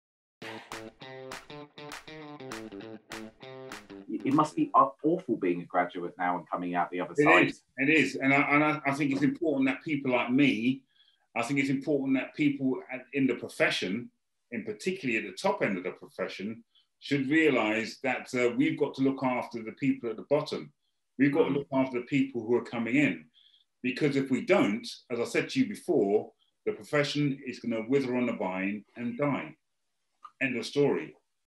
Clip #5 - Special Episode of The Career Success Podcast w/ Shaun Wallace & Jason Connolly
In this special episode of the Career Success podcast we speak to ITV’s, The Chase, Shaun Wallace. Shaun was the first ever Chaser Globally.  This episode is uplifting and inspiring, this is the story of one man's journey as a son of immigrants to fulfilling his own dream. This episode is made in collaboration with The Law Society.

“I think it’s important that people like me; I think it’s important that people in the profession, particularly at the top end of the profession, should realise that we’ve got to look after the people at the bottom. We’ve got to look after the people who are coming in. Because, if we don’t as I said to you before; the profession is going to wither on a vine and die. End of story.”